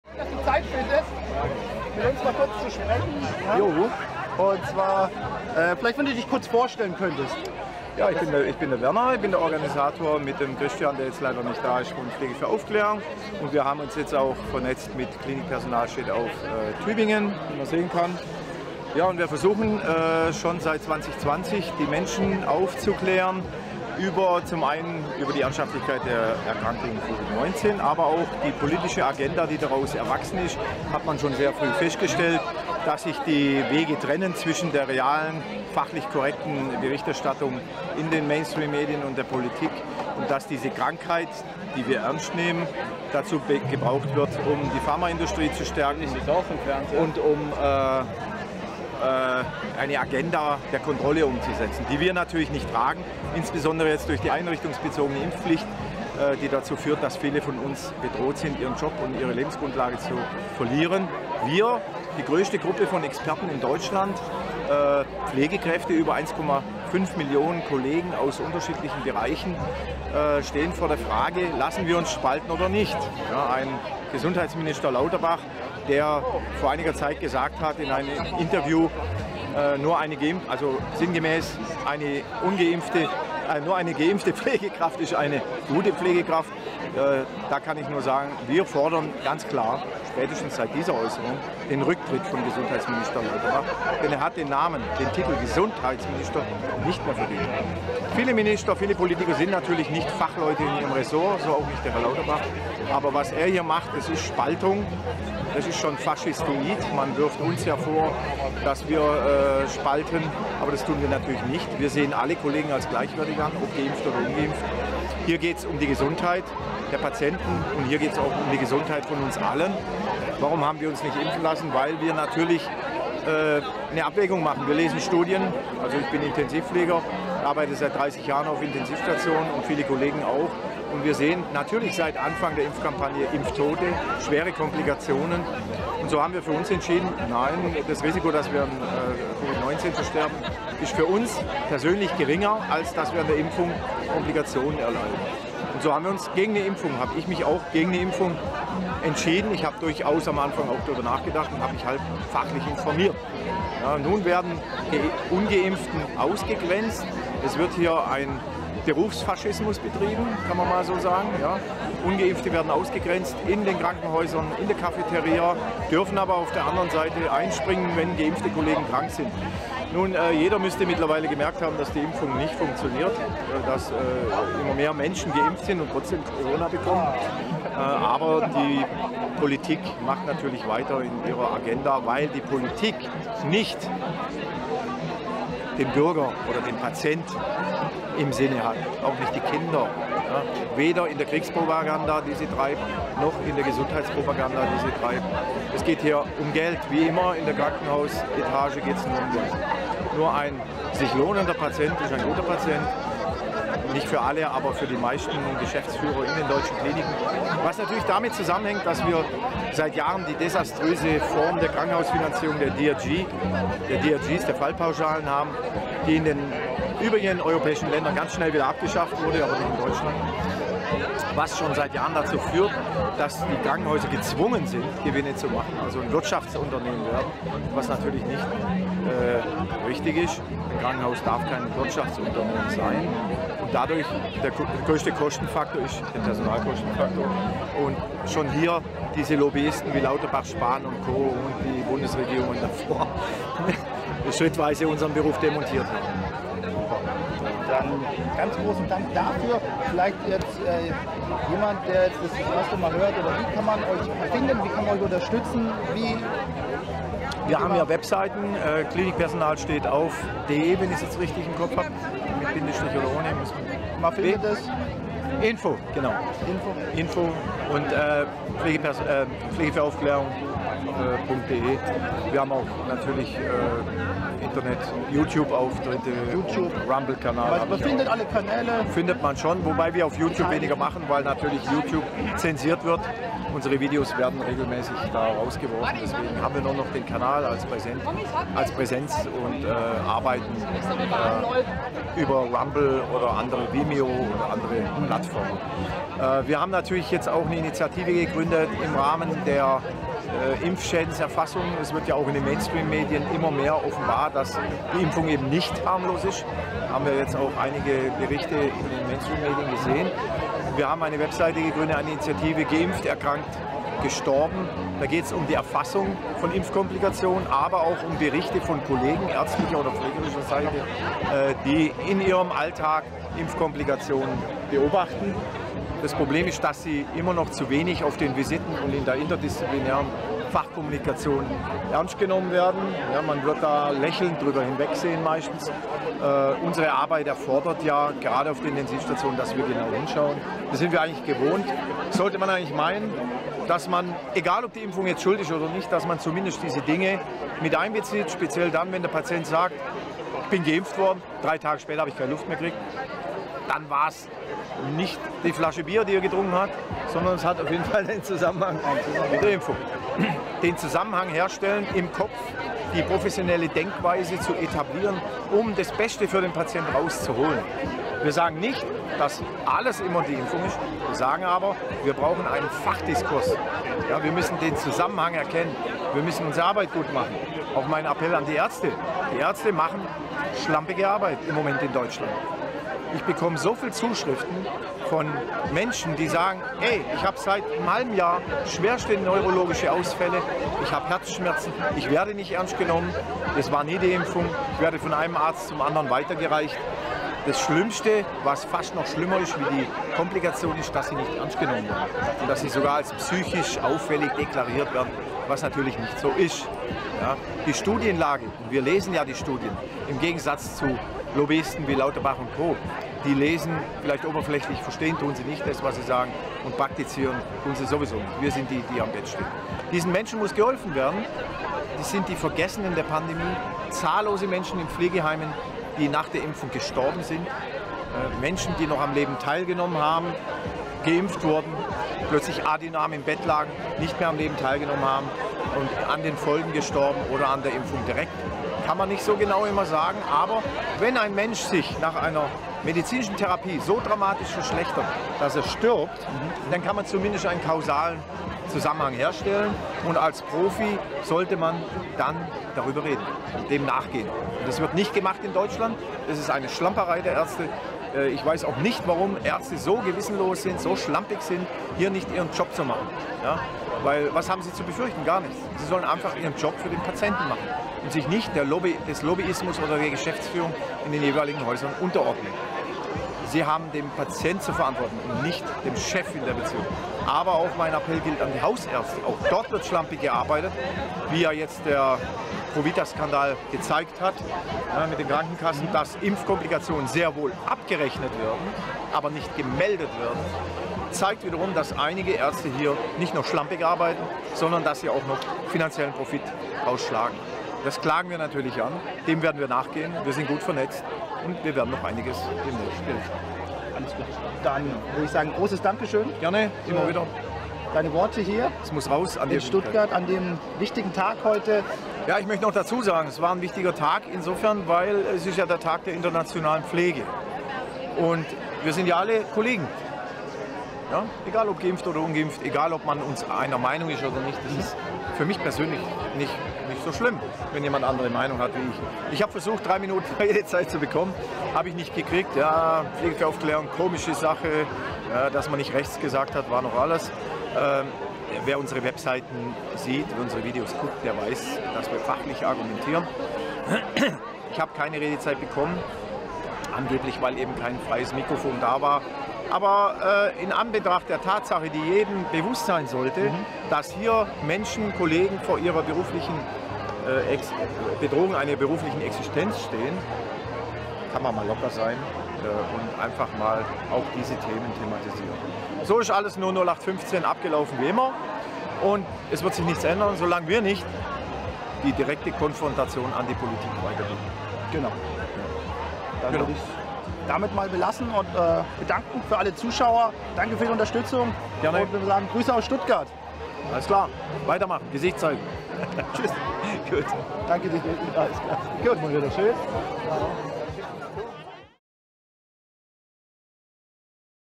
Stuttgart 12.05.2022 - Interview mit Werner von "Klinik Personal steht auf"
Am Internationalen Tag der Pflege haben wir ein Interview geführt mit Werner von Klinik Personal steht auf auf dem Stuttgarter Schloßplatz. Durch das Interview führte Matthias (Schwäble On Tour).